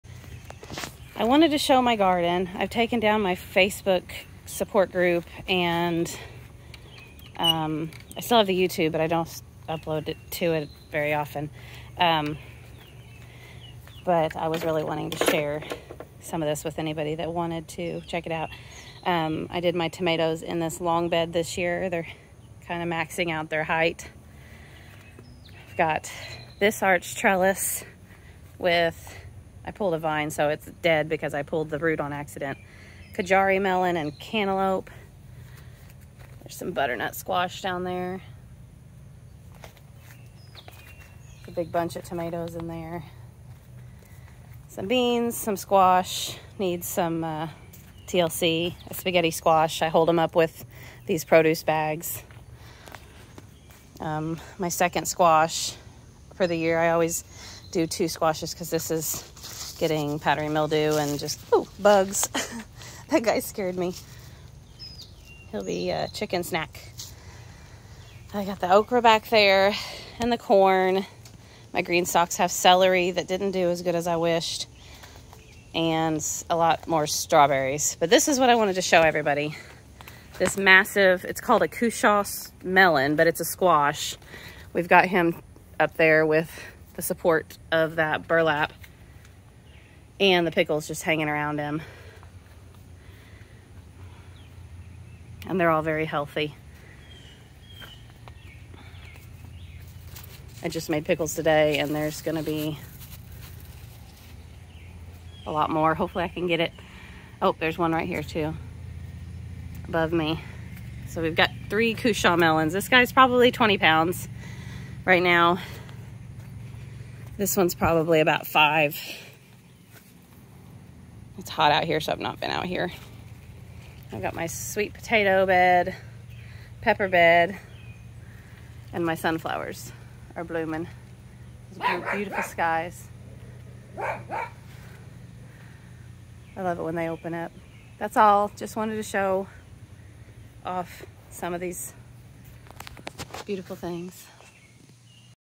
July Garden Tour | Cushaw squash on an arched cattle panel trellis
Just an update on how the gardens doing this summer.